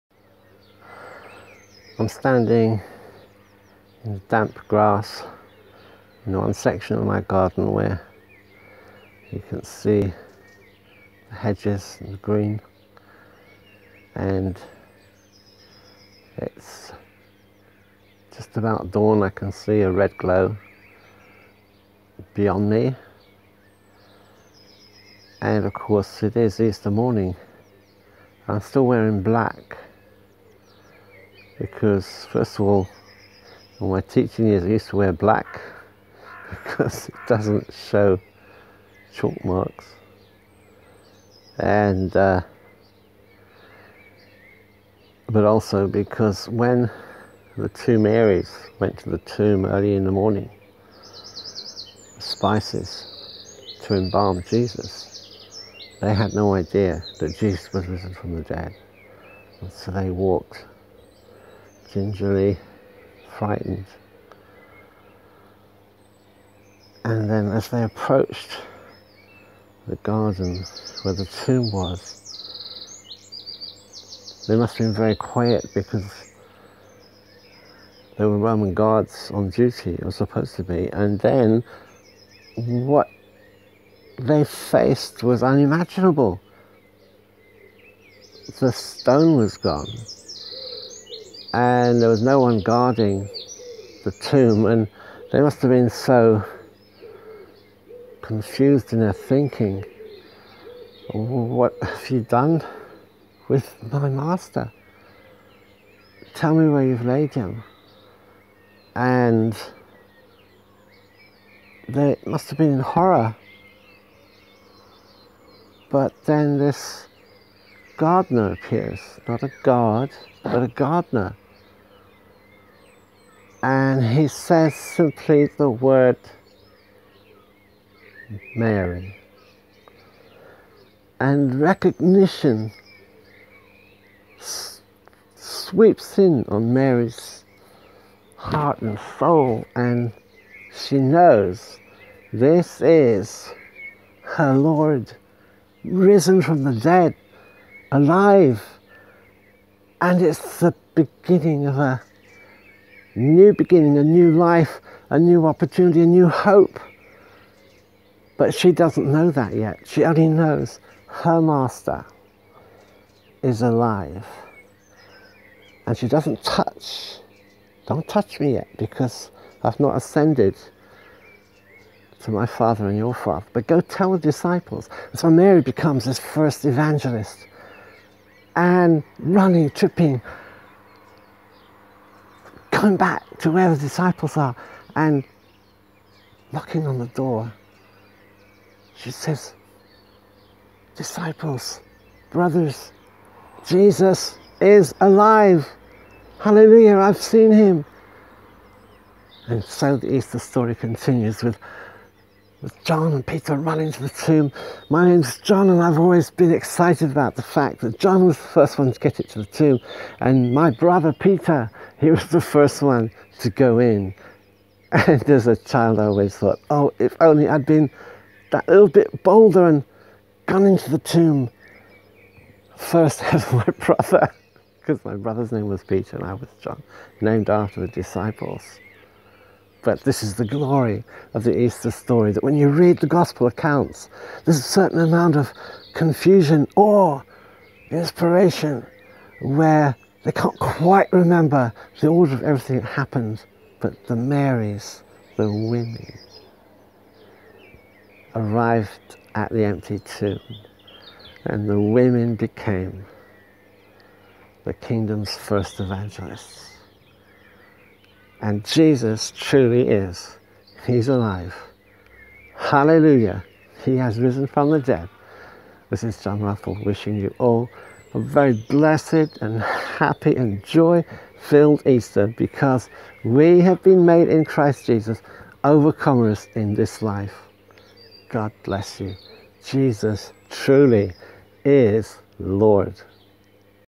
012 EASTER DAY: An Easter Morning Narrative w/John Ruffle
I endeavour to re-live that early morning journey to the tomb in this short narrative. Through the years, I've written several Easter-related cameo plays that have been performed in both America and here, in England. During Holy Week, I'd been working on updating some of my previous work, but there was a snag: the quarantine conditions prevailing in the UK at present meant I only had one female player - my wife. I'd considered trying to pull together a cast from our charismatic prayer group via Zoom, but I could see that too much human sweat was involved, so the project ground to a halt and I enjoyed my sleep, without a clue of what I'd do for today's piece when I awoke.

In the event, early this morning, before even turning on my coffee maker (!) I went outside to our small back garden and recorded this piece to camera in one take. I offer these flawed crumbs first in honour and love to our Risen Lord, and in doing so, also to you, dear, beloved viewer, wherever and whenever you are. Jesus is Lord, and He is Risen indeed. Hallelujah! - John E. Ruffle, Easter Sunday, April 12, 2020.

The early morning Resurrection encounters in the four Gospels:
St. Matthew 28:1-10
St. Mark 16:1-12
St. Luke 24:1-12
St. John 20:1-18

THOUGHTS ON THE GOSPEL NARRATIVES:
Jesus appears first to Mary Magdalene, out of whom he had driven seven demons. (Mark 16:9). All four Gospel accounts mention the presence of one or two angels (Matt. 28:2; Mark 16:5; Luke 24:4,5; John 20:10-12). However, Mary Magdalene's encounter with "The Gardener" (which I draw-upon in today's Easter Sunday narrative) is related only by St. John, who writes with such depth of intimacy and pathos, that his words paint a picture that reverberates to this very Resurrection morning, as if it had just occurred (St. John 20:10-18). The Gospels carry points of difference in their re-telling of the Resurrection. Far from invalidating the evidence however, does this not surely confirm the overwhelming truth of this event? And here's why: had the disciples been seeking to create a new religion, then surely they would have been working hard to ensure that each narrative lined-up with each other perfectly? But no, rather the narratives are rather like a jig-saw puzzle, yet the main even is there, bold, standing clear; now and for all eternity: the stone has been rolled away. The tomb is empty. And just as the meek and humble Jewish virgin, Mary, was the woman God chose to be the vessel of his birth into humanity, so again God chooses women to be the first witnesses of Jesus' resurrection. How wonderful! Let me never hear that women don't have a place in the Church of Jesus Christ: they have all along! And together with the women, we proclaim, Jesus has risen from the dead. He is alive! He has risen indeed. Hallelujah!  - John E. Ruffle.

CREDITS:
Background chorus is sung by the birds, appearing live, courtesy of their creator.